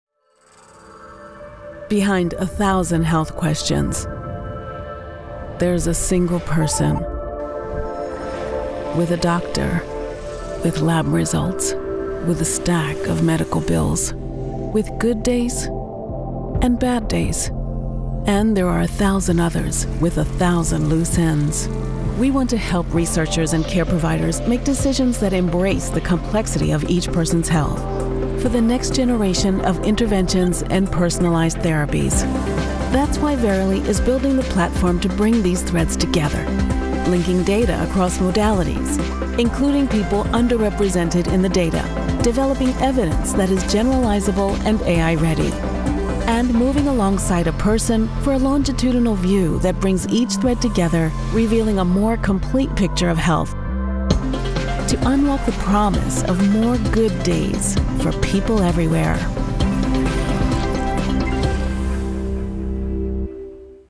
The Verily Platform
Discover how Verily’s platform is designed to enable more personal, precise, AI-driven care experiences and workflows, while driving down costs.